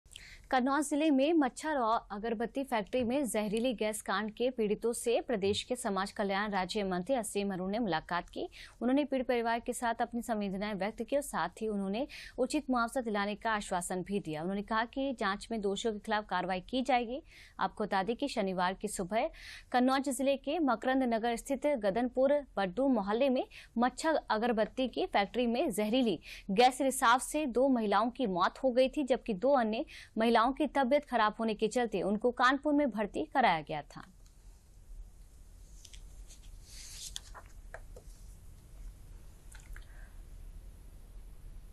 कन्नौज में जहरीली गैस कांड के पीड़ितों से समाज कल्याण मंत्री असीम अरुण ने की मुलाकात ।
कन्नौज जिले में मच्छर अगरबत्ती फैक्ट्री में जहरीली गैस कांड के पीड़ितों से प्रदेश के समाज कल्याण राज्य मंत्री असीम अरुण ने मुलाकात की। उन्होंने पीड़ित परिवार के साथ अपनी संवेदनाएं व्यक्त की साथ उन्होंने उचित मुआवजा दिलाने का आश्वासन दिया। उन्होंने कहा कि जांच में दोषियों के खिलाफ कार्रवाई की जाएगी। आपको बता दें कि शनिवार की सुबह कन्नौज जिले के मकरंद नगर स्थित गदनपुर बड्डू मोहल्ले में मच्छर अगरबत्ती की फैक्ट्री में जहरीली गैस रिसाव से दो महिलाओं की मौत हो गई थी जबकि दो अन्य महिलाओं की तबियत खराब होने के चलते उनको कानपुर में भर्ती कराया गया था।